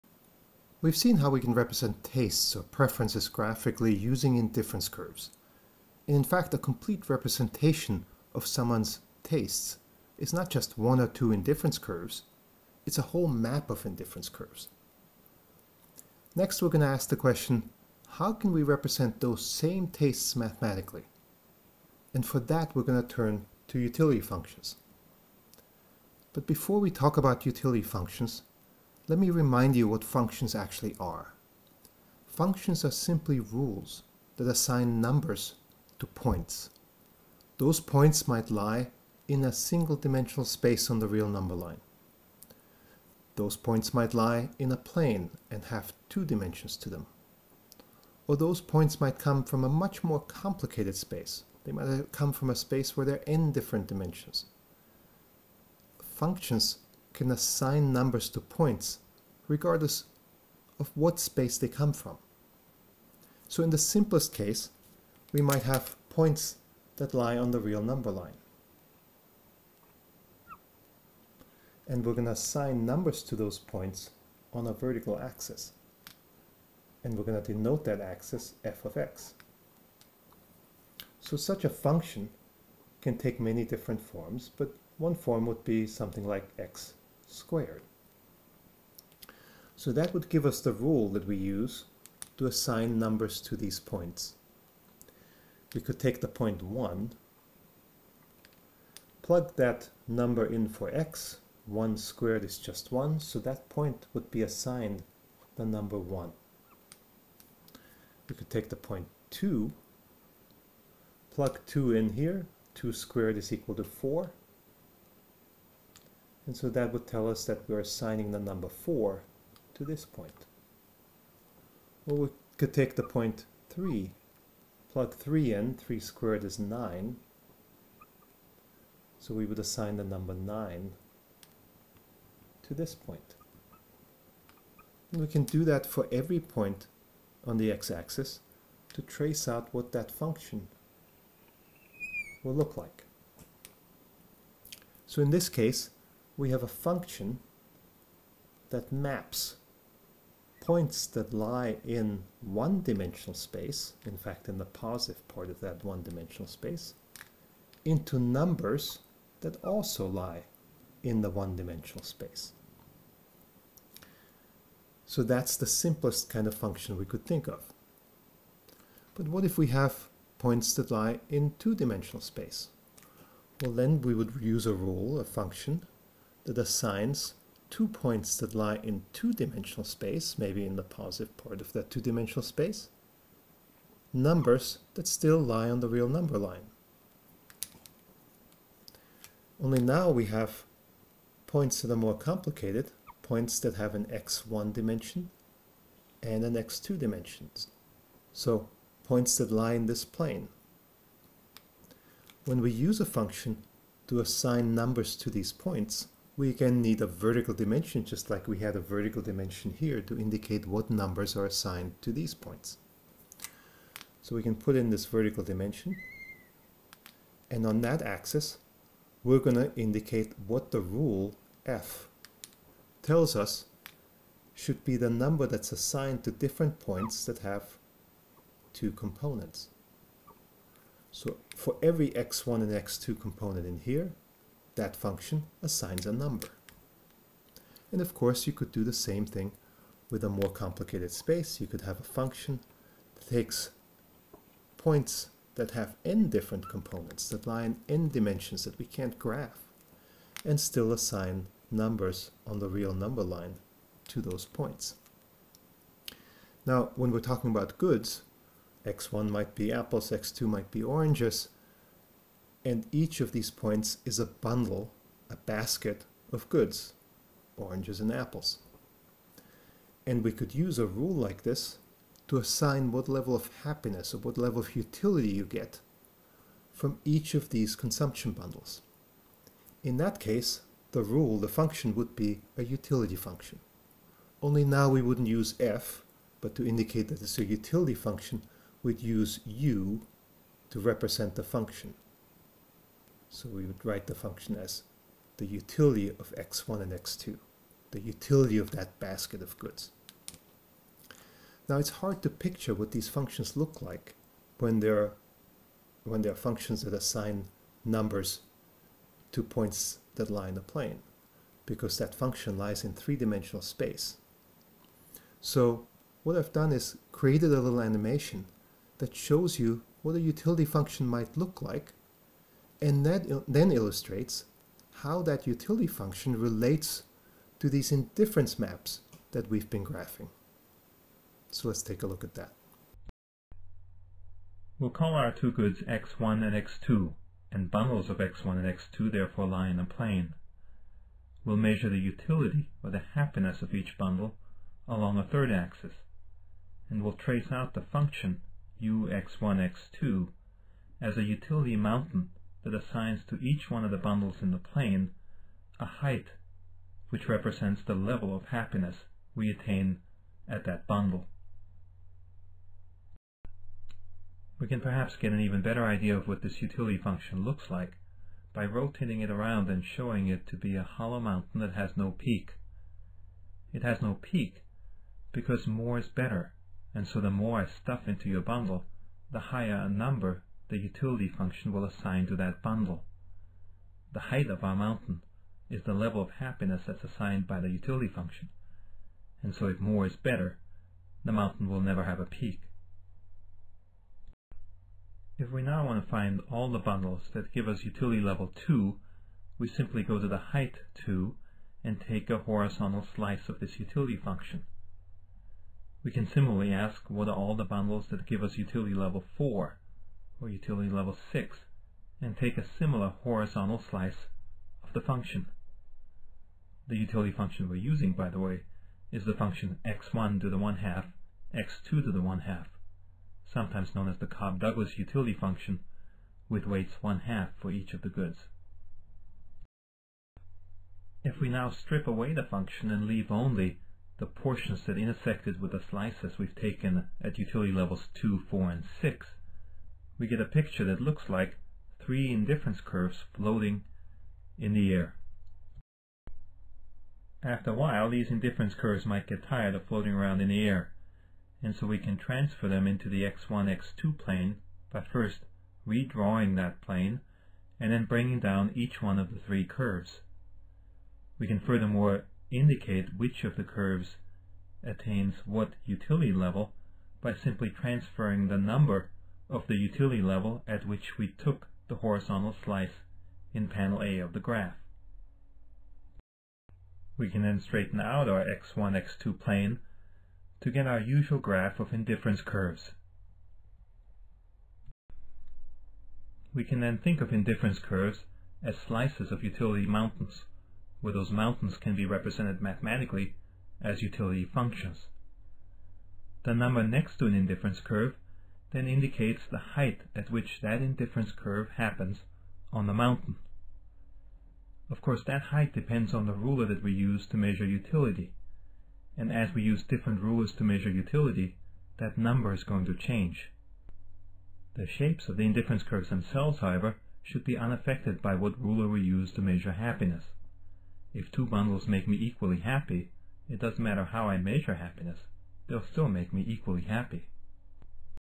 4.1.Utility Functions and Indifference Maps
This video relates the concept of a utility function to an indifference map that is represented by that function.